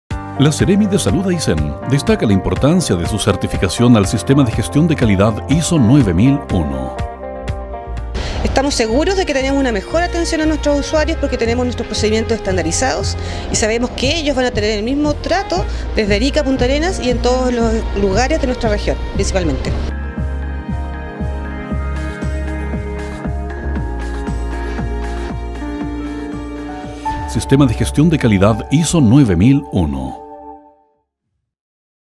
Sistema de Gestión Calidad Desratización
Claudia Alvarez, Encargada de Unidad de Zoonosis, Seremi de Salud
Sistema de Gestión de Calidad ISO 9001/2015